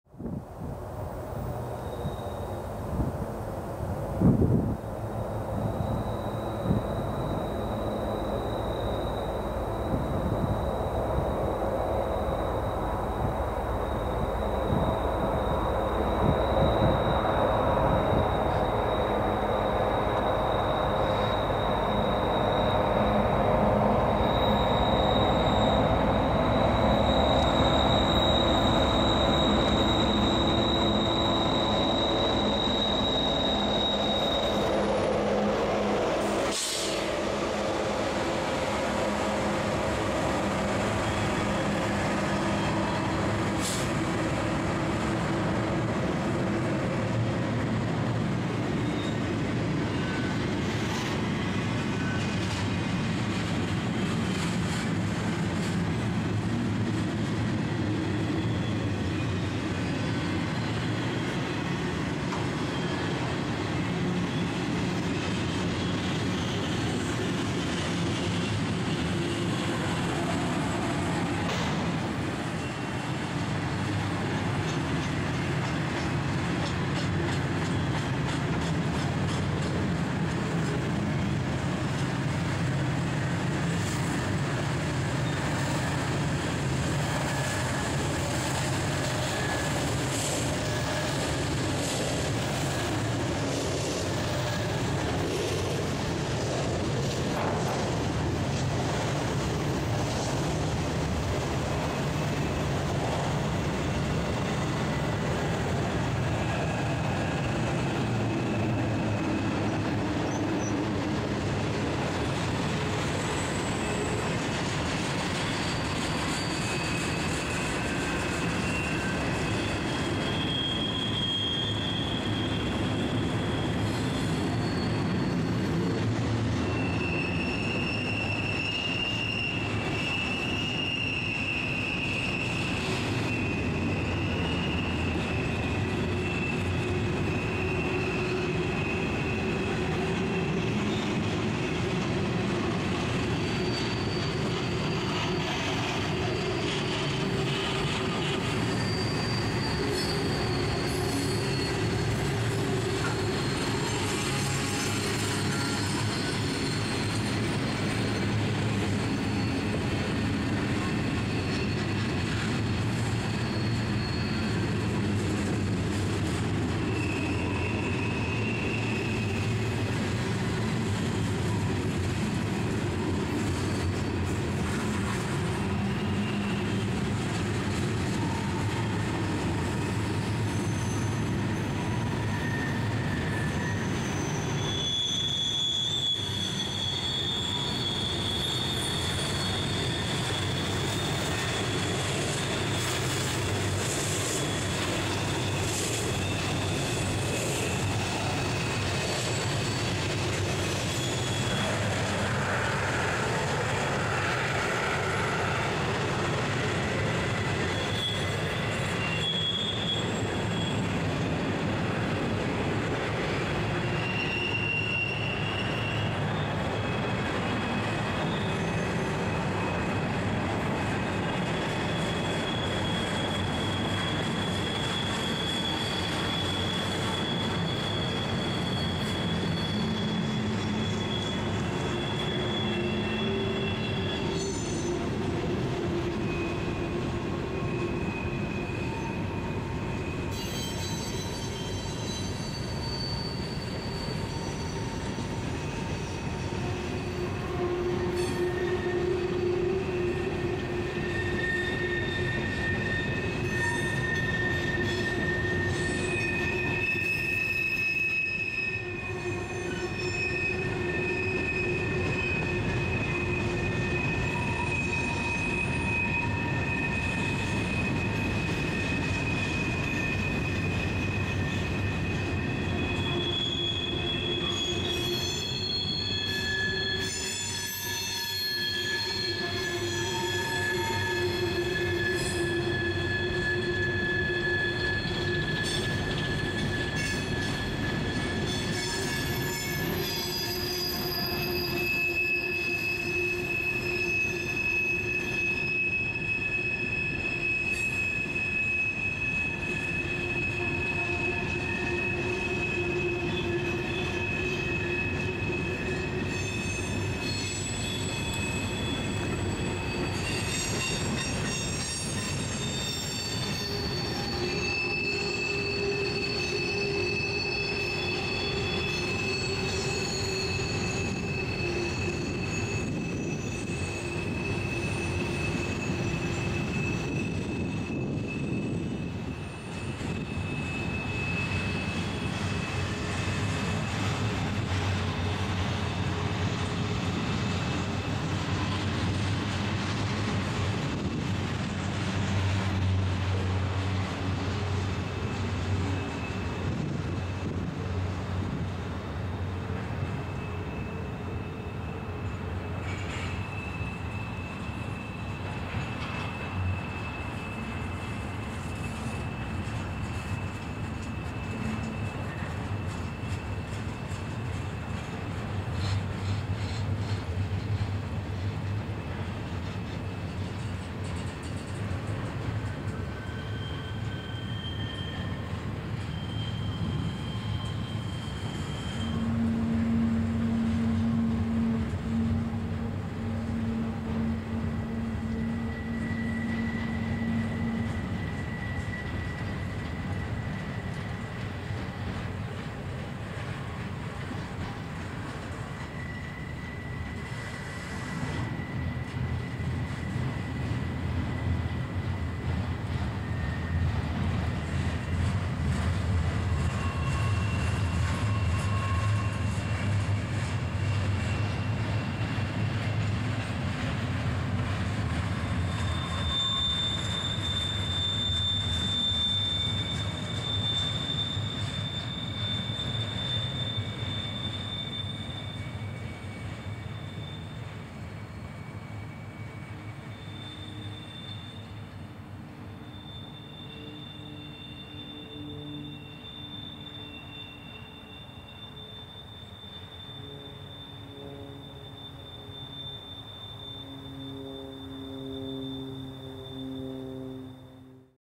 A Triple at Bennington Curve
20R descends track 1, 593 is on track 2 and 23M is on main 3. 4/21/18